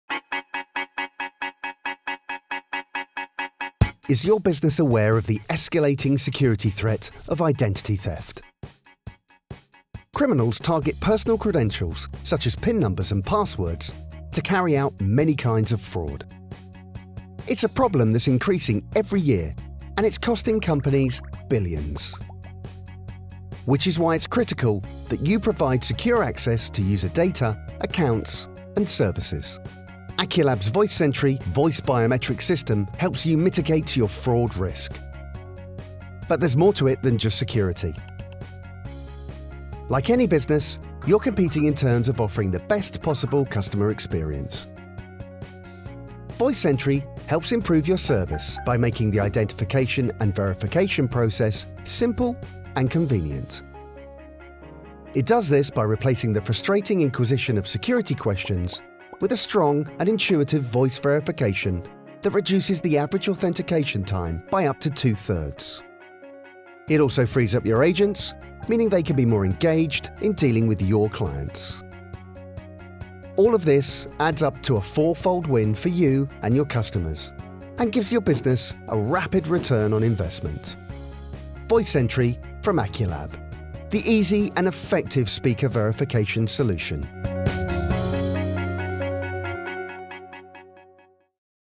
VoiSentry - Biometric speaker verification from Aculab
VoiSentry APIs enable developers to add the enhanced security and user convenience of biometric voice authentication to their customer contact applications.

Now, your business customers can replace frustrating, time-consuming verification processes simply by allowing their users, clients or subscribers to authenticate with their voice.